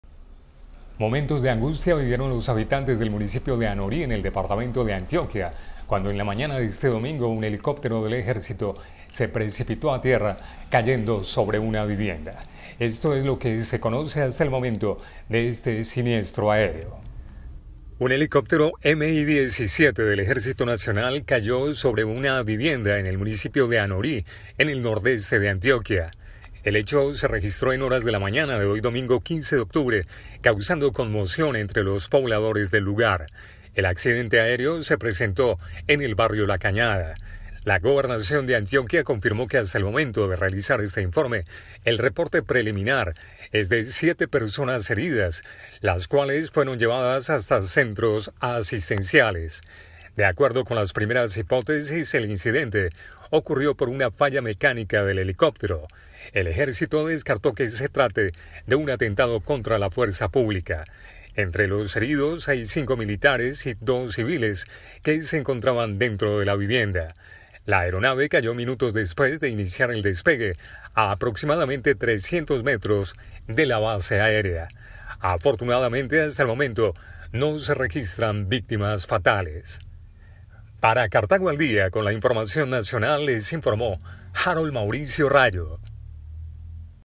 HELICÓPTERO DEL EJÉRCITO SE ACCIDENTÓ EN ANORÍ ANTIOQUIA
Noticias de Colombia